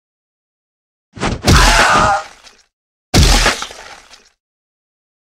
AXE KILL sticknodes animation
a simple animation about the incoming stick empires the movie